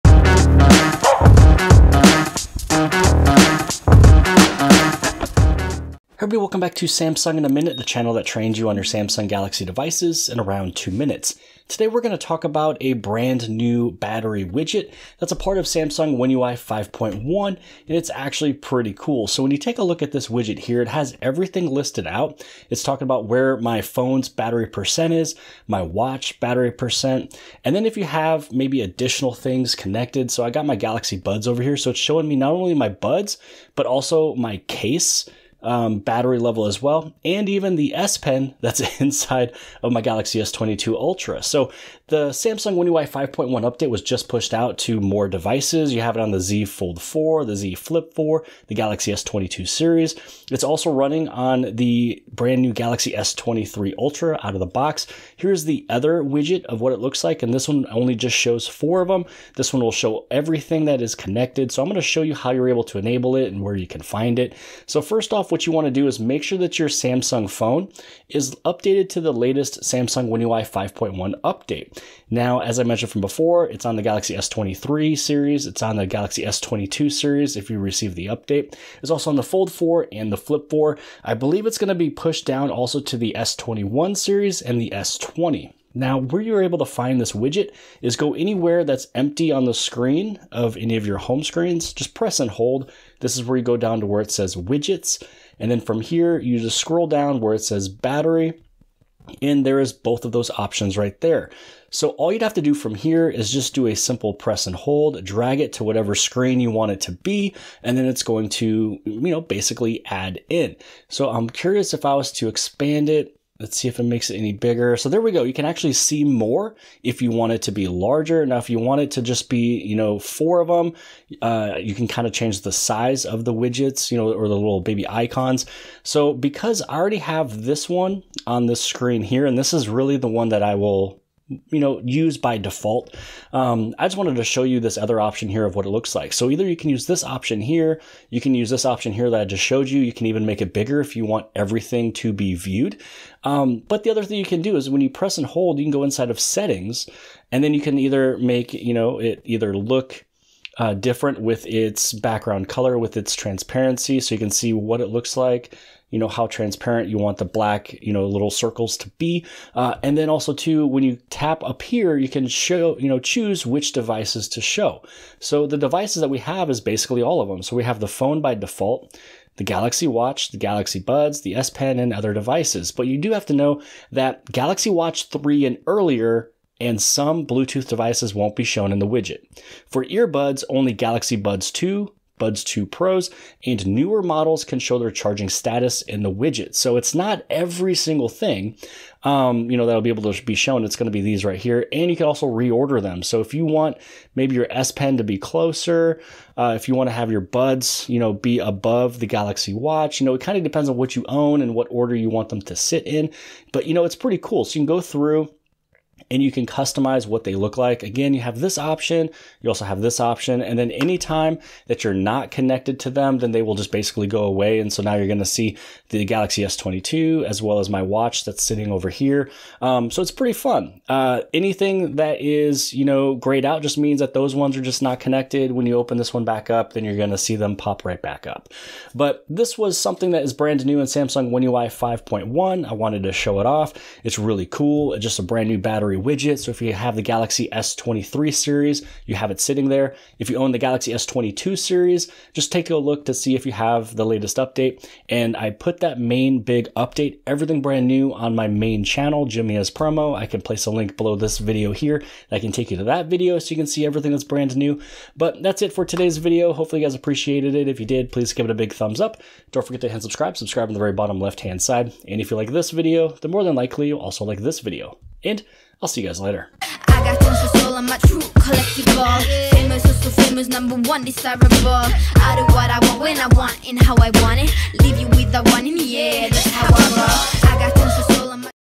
Amazing New Battery Widget - Samsung One UI 5.1
Here at Samsung In A Minute, We get to the point in around 2 minutes. In Today's Video, we will be taking a look at the new Battery Widget found on your Samsung with Samsung One UI 5.1.

My Galaxy S22 Ultra Protective Case of Choice